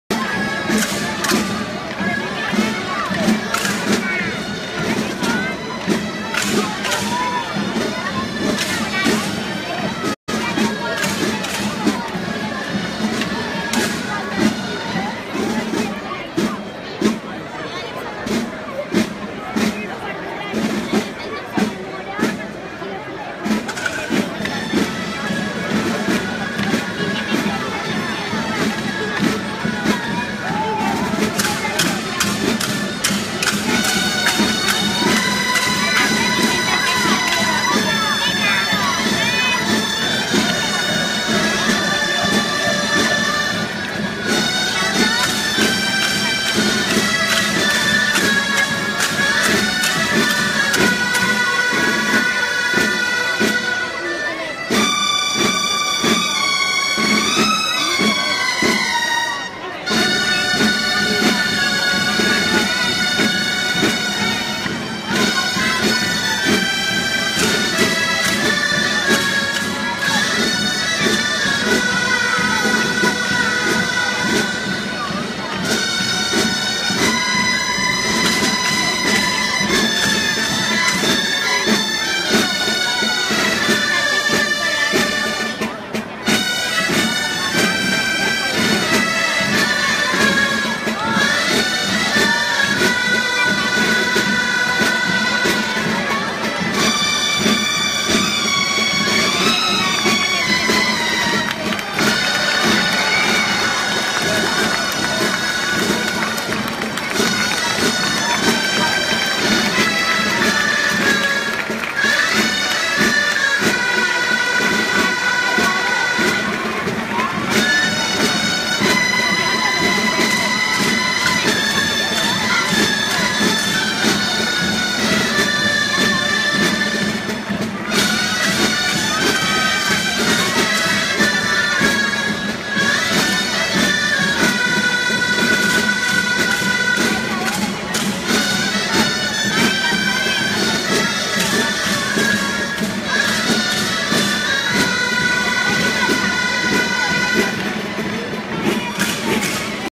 Cinc d'Oros (intent) - Nova Muixeranga - Mare de Déu de la Salut d'Algemesí - 08/09/2017
Intent de la figura del Cinc d'Oros de la Nova Muixeranga d'Algemesí en la Processó de Volta General de les Festes de la Mare de Déu de la Salut d'Algemesí el divendres 8 de setembre del 2017.